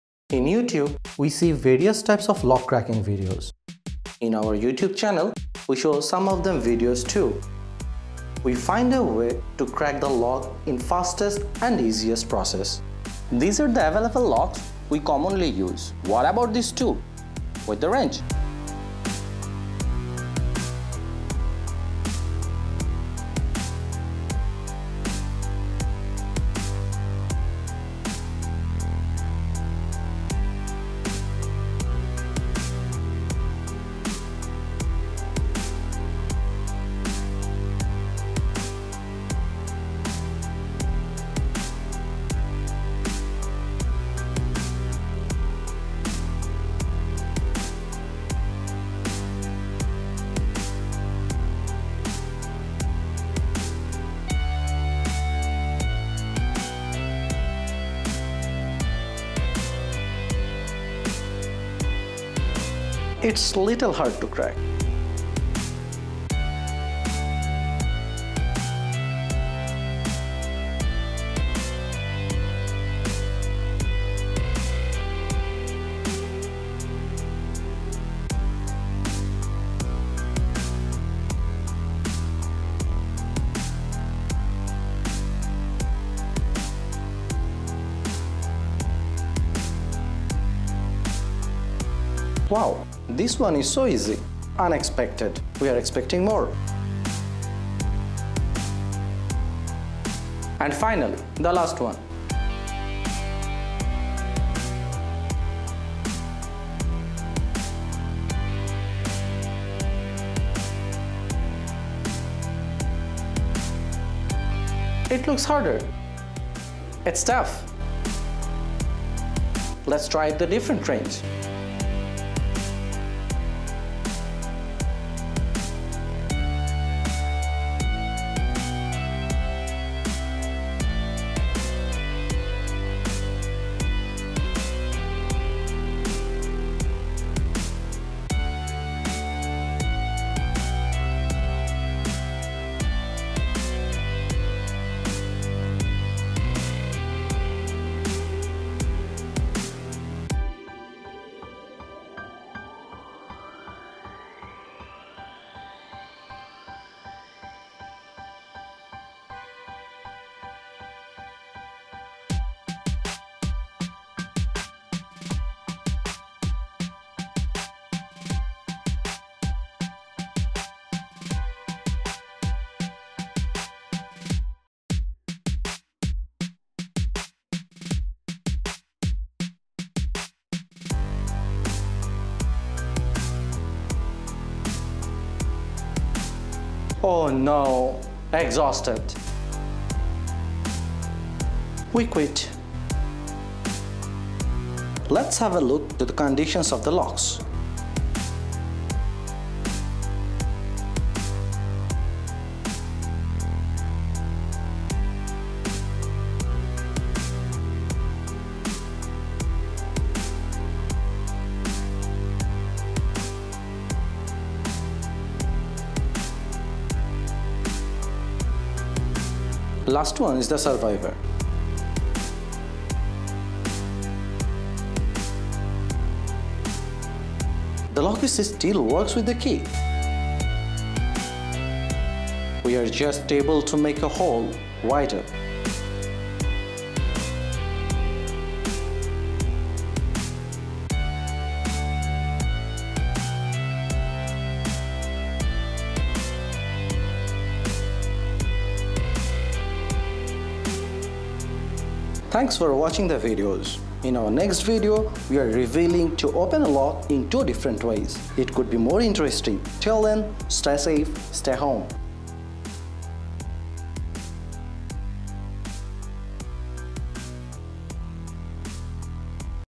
Fastest Way to Break Lock !!
Hi viewers, getting bored in staying home.

We are here for you with a new video named, "Fastest Way to Break Locks". We picked different common padlocks. Did different experiments, and got a best way to lock hack as a life hack tool.

In our upcoming videos, we will show you 3 ways to open a lock by using combination lock. We will come forward with new new experiments.